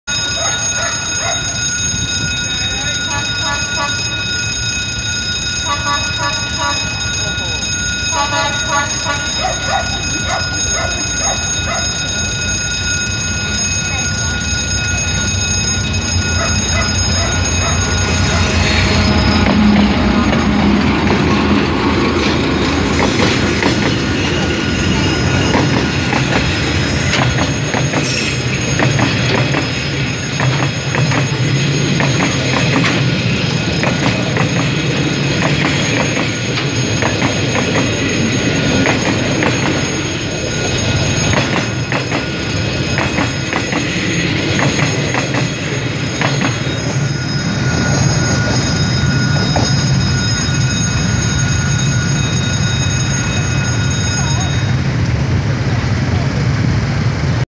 [D12E-622] Tàu hàng A4801/9802 thông qua Km12+650 vành đai Văn Điển - Bắc Hồng | Freight train VNR
Date: 23/04/2022
Location: Km12+650 vành đai Văn Điển - Bắc Hồng (chắn KĐT Handiresco)
Video included: Locomotive D12E-622 (VNR)
                             Freight train A4801/9802 (VNR)